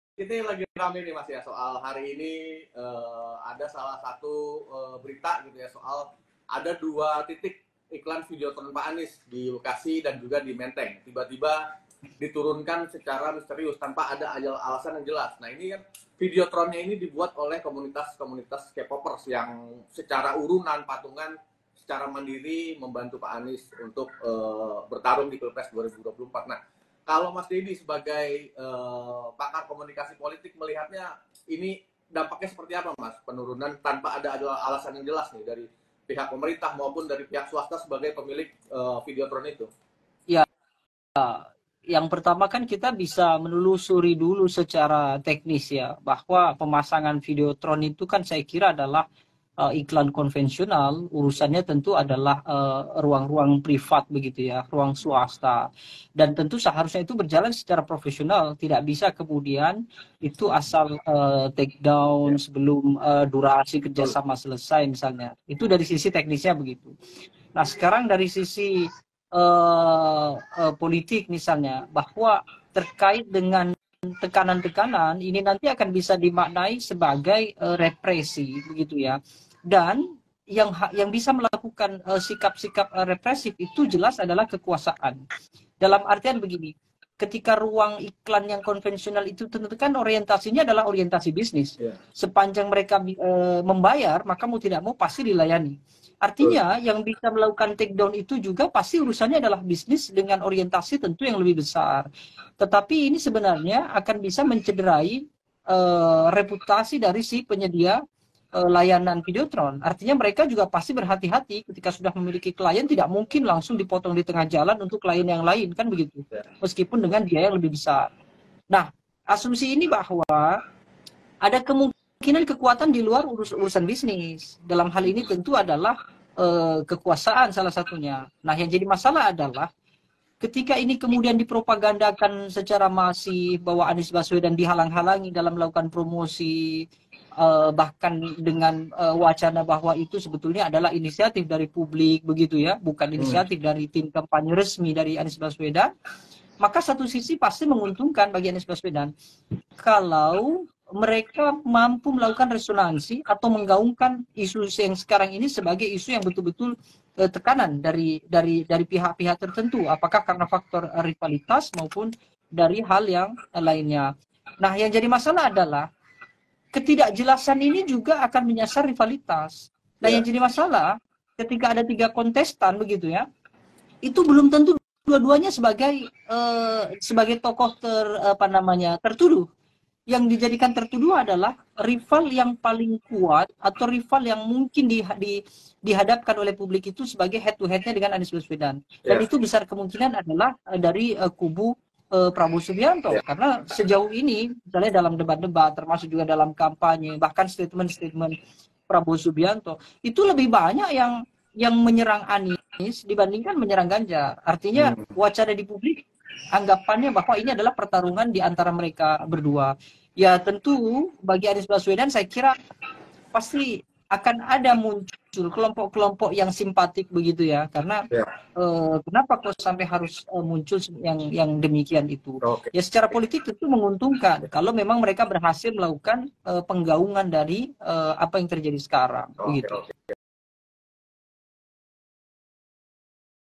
IKLAN VIDEOTRON ANIES MENDADAK DITURUNKAN, INI KATA PENGAMAT
Videotron yang memperlihatkan Capres nomor urut 01, Anies Baswedan yang dilengkapi dengan animasi Korea diturunkan sebelum masa pemasangan selesai. Videotron yang di desain secara kolaboratif antara Anies Bubble dan Olppaemi Project itu, mulanya terpasang di depan Grand Metropolitan Mal Bekasi dan Graha Mandiri Jakarta.
Direktur Eksekutif IPO, Dedi Kurniasyah mengatakan bahwa iklan tersebut tentunya harus bisa berjalan secara professional dan tidak bisa diturunkan secara sembarangan sebelum durasi kerja sama selesai, sebab pemasangan videtron adalah iklan konvensional yang berurusan dengan ruang privat dan pihak swasta.
Dedi Kurniasyah turut melihat fenomena tersebut dari sisi politik, bahwa aksi ini bisa dimaknai sebagai represi dan yang bisa melakukan sikap represi tersebut adalah kekuasaan. Iklan konvensional termasuk orientasi bisnis, sepanjang pihak kedua membayar, maka pihak pertama harus melayani. Dedi menilai bahwa yang bisa melakukan penurunan videotron adalah bisnis dengan orientasi yang lebih besar. Namun Dedi mengatakan bahwa aksi ini juga akan menurunkan reputasi penyedia layanan iklan tersebut, serta adanya kemungkinan kekuatan di luar urusan bisnis, dalam hal ini adalah adanya kekuasaan.
Selanjutnya, Dedi menuturkan ketidakjelasan penurunan videtron juga akan menyasar kepada lawan dari paslon lain. Dedi menilai kemungkinan paslon yang akan tertuduh adalah paslon yang paling kuat atau lawan yang dihadapkan oleh publik sebagai lawan ternesar dari Anies Baswedan.